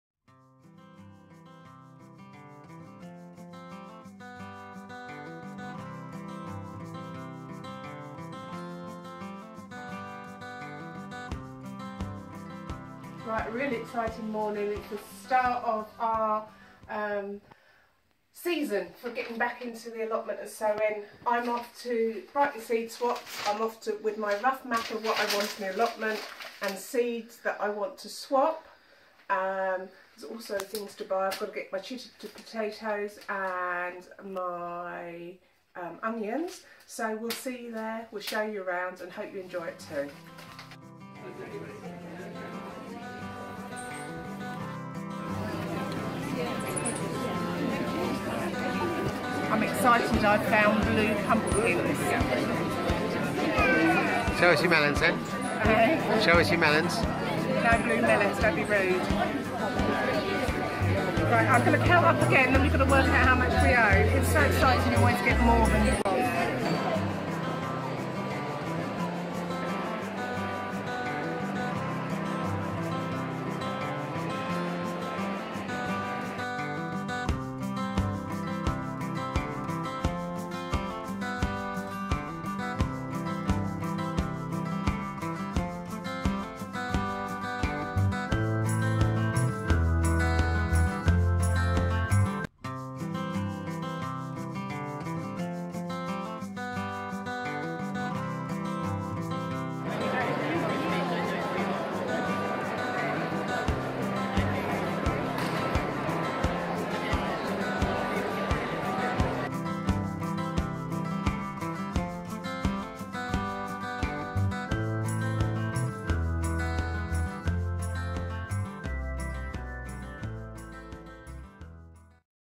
Ginger Gardening S2 Seedy Sunday Special 05 02 16
A trip to Seedy Sunday to get the new gardening year up and running.